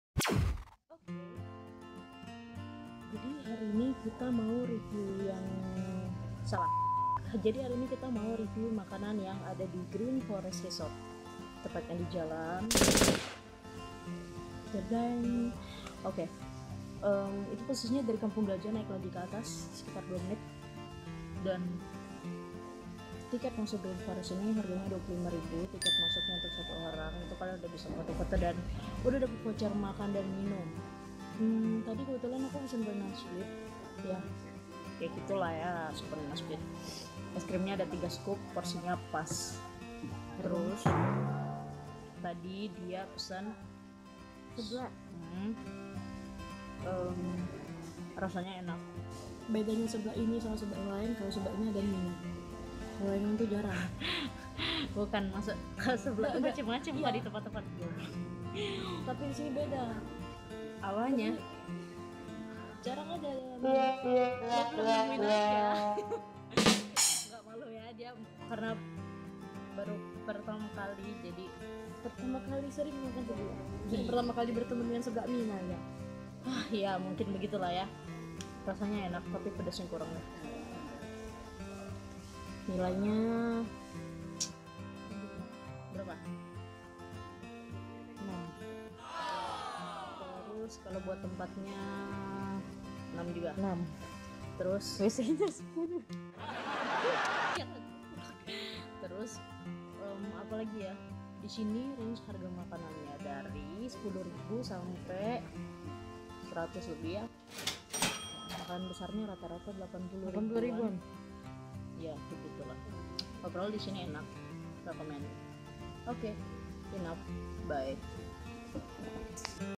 Review The Green Forest Resort Bandung
Ok, kali ini kita berdua bakal Review di salhsatu restoran di Bandung yaitu The Green Forest Resort, Yay!🤗